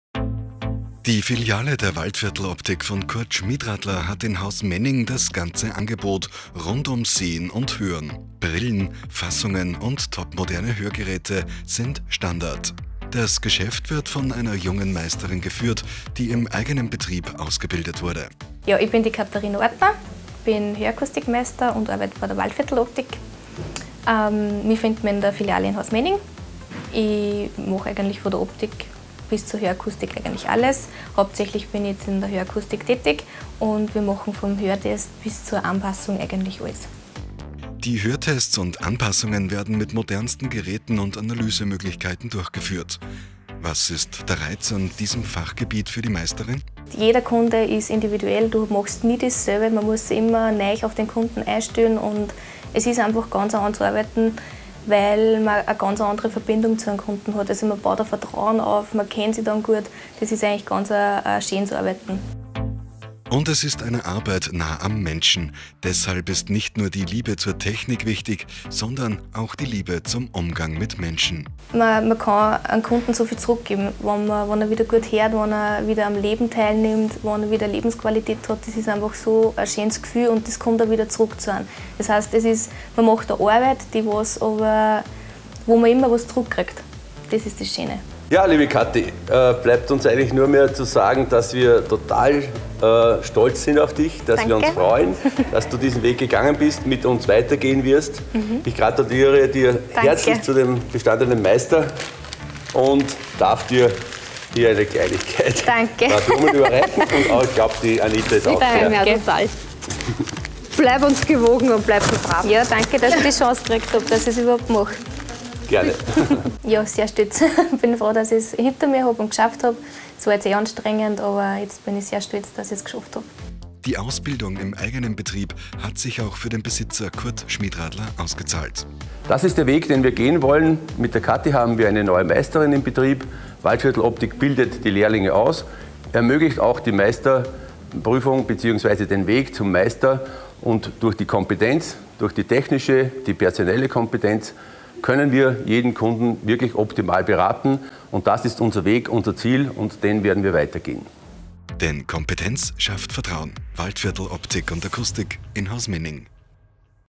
Waldviertel Optik und Akustik gratuliert Katharina Ortner
Die Filiale der Waldviertel Optik von Kurt Schmidradler hat in Hausmening das ganze Angebot rund um Sehen und Hören. Brillen, Fassungen und topmoderne Hörgeräte sind Standard. Das Geschäft wird von einer jungen Meisterin geführt, die  im eigenen Betrieb ausgebildet wurde.